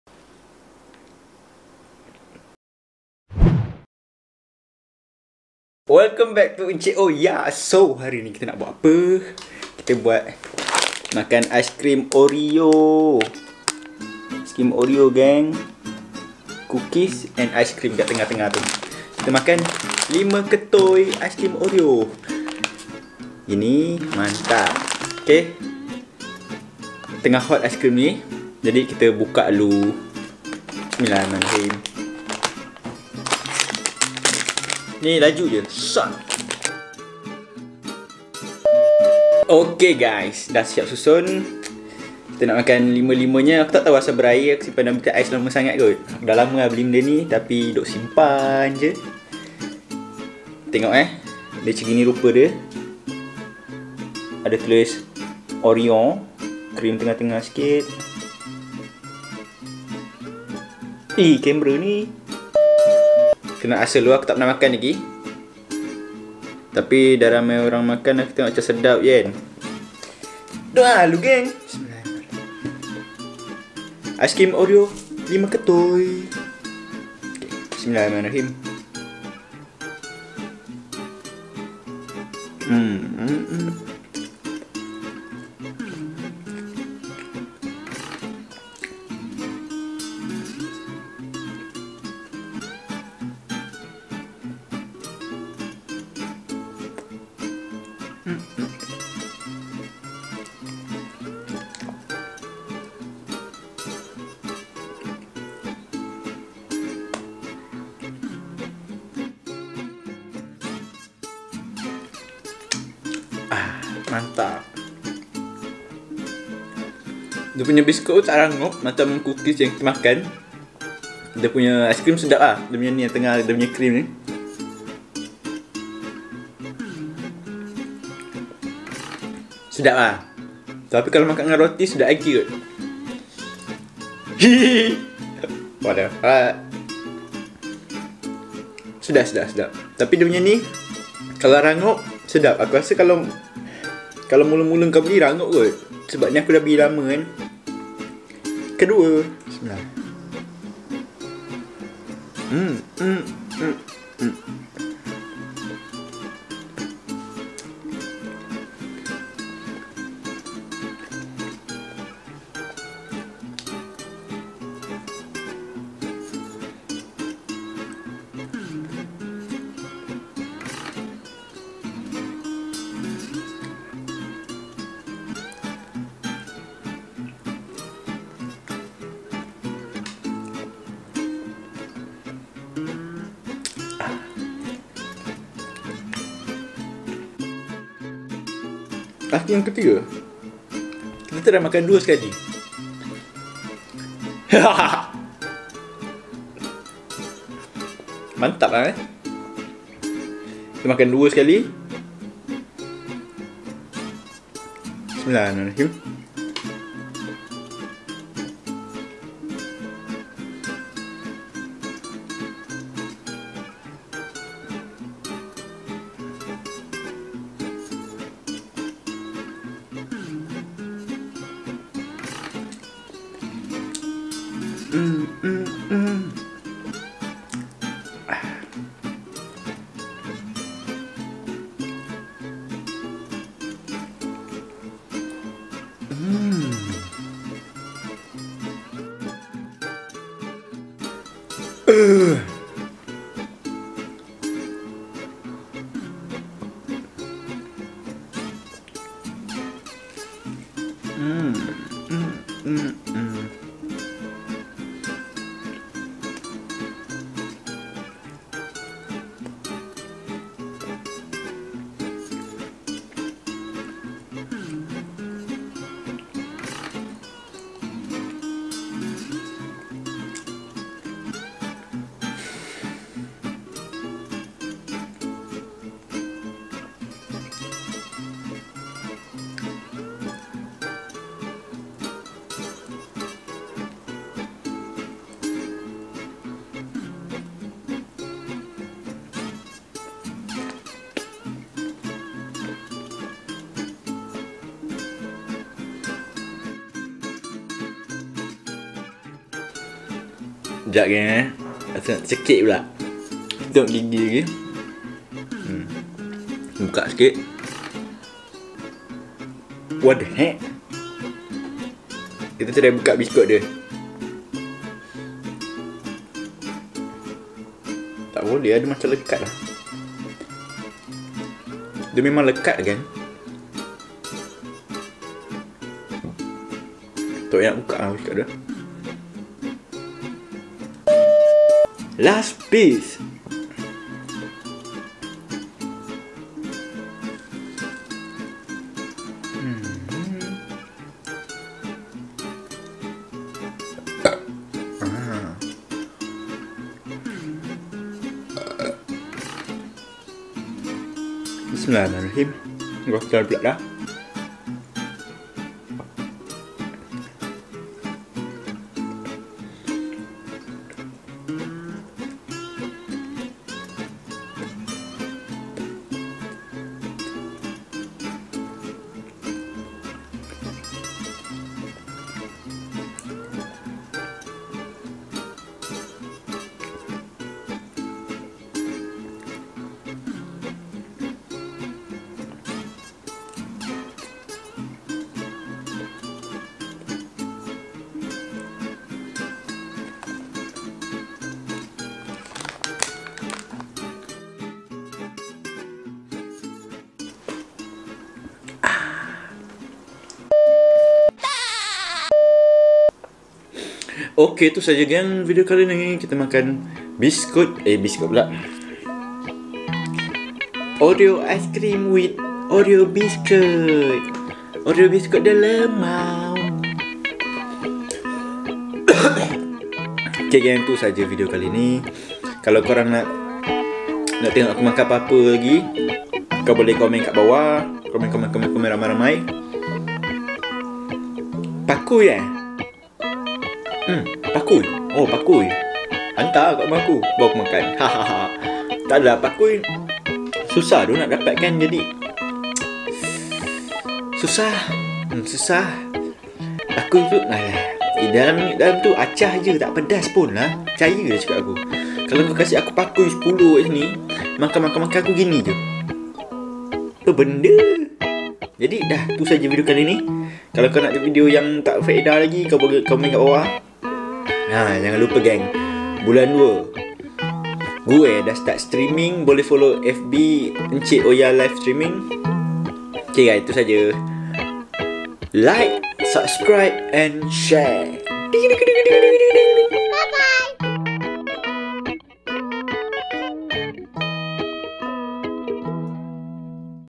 MAKAN 5 KETUL ICE CREAM OREO SEKALIGUS !? (KENA TENGOK WOI)
Wokey geng harini makan eskrim oreo pulerks !! 5 ketui woii !! silakan tengok mukbang eskrim ni ye. THANKS !

Taknak Takpe :)

Socmed ;
ig: encikoyaa (for any business requirement please dm)